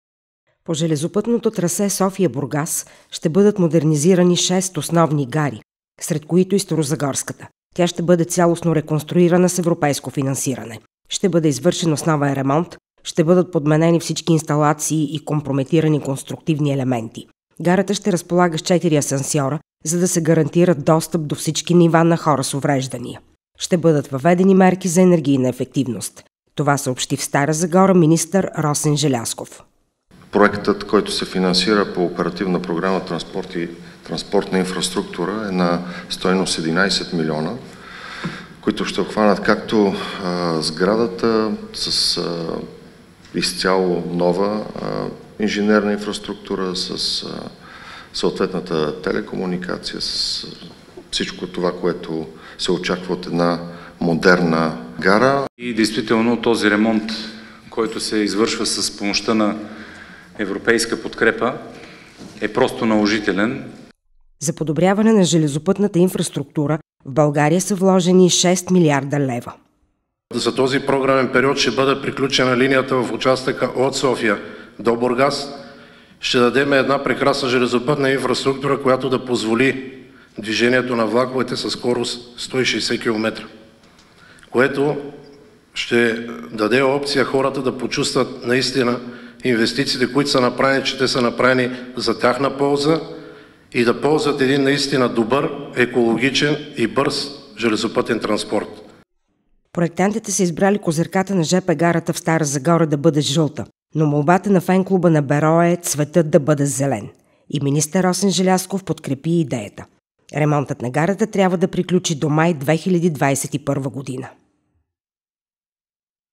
Старозагорската гара ще бъде изцяло обновена
По железопътното трасе София – Бургас ще бъдат модернизирани шест основни гари, сред които и старозагорската. Тя ще бъде цялостно реконструирана с европейско финансиране. Ще бъде извършен основен ремонт, ще бъдат подменени всички инсталации и компрометирани конструктивни елементи.

Гарата ще разполага с четири асансьора, за да се гарантира достъп до всички нива на хора с увреждания. Ще бъдат въведени мерки за енергийна ефективност, съобщи в Стара Загора министърът на транспорта, информационните технологии и съобщенията Росен Желязков.
„Проектът, който се финансира по оперативна програма „Транспорт и транспортна инфраструктура”, е на стойност 11 млн. лева и ще обхване сградата с изцяло нова инженерна инфраструктура, със съответната телекомуникация. Ще бъде направено всичко, което се очаква от една модерна гара” – каза Росен Желязков.

„Този ремонт, който се извършва с помощта на европейска подкрепа, е крайно наложителен, защото сградата е в лошо състояние, има много течове и оплаквания от хората” – каза кметът Живко Тодоров.

За подобряване на железопътната инфраструктура в България са вложени 6 млрд. лева. „В този програмен период ще бъде завършена линията в участъка от София до Бургас. Ще осигурим една прекрасна инфраструктура, която да позволи движението на влаковете със скорост 160 км, което ще даде възможност на хората да почувстват инвестициите, които са направени, и да ползват екологичен и бърз транспорт” – коментира генералният директор на Национална компания „Железопътна инфраструктура” Красимир Памукчийски.

Проектантите се избрали козирката на жп гарата в Стара Загора да бъде жълта, но молбата на фенклуба на „Берое” е цветът да бъде зелен. Министър Росен Желязков подкрепи идеята. Ремонтът на гарата трябва да приключи до май 2021 г.

ilko gruev